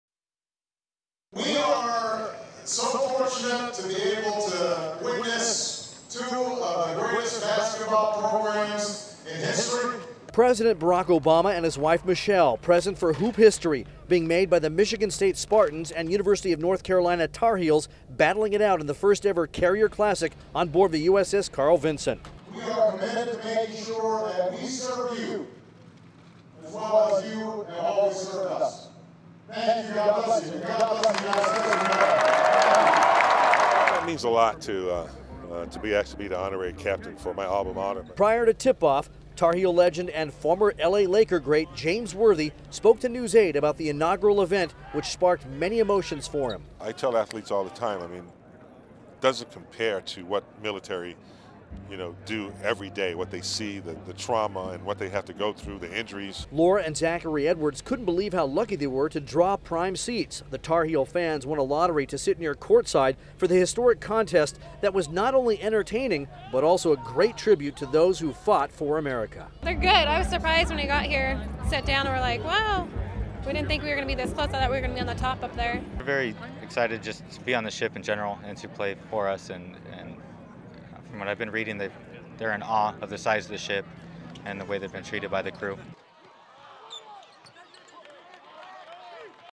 President Obama, First Lady attend Carrier Classic aboard USS Carl Vinson in 2011
President Barack Obama visited San Diego to watch North Carolina face Michigan State in the inaugural Carrier Classic game aboard the USS Carl Vinson in Nov 2011.